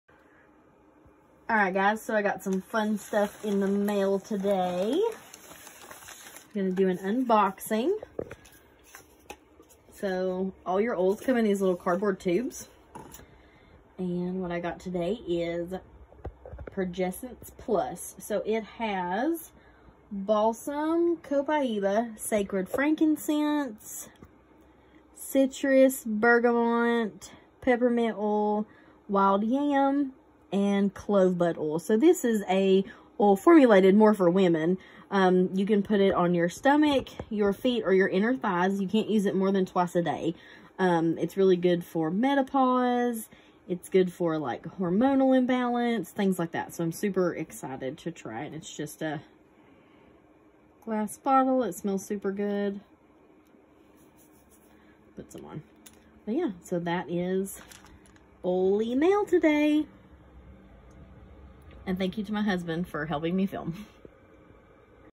Young living Short oily mail video.
In this video I do a short unboxing of the young living Pro plus oil.
Link to my young living account.